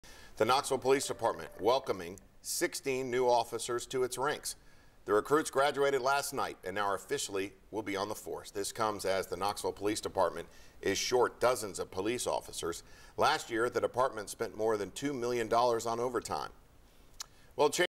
KPD welcomes 16 new officers to its ranks
Knoxville Police is short dozens of police officers and is spending millions on overtime because of it.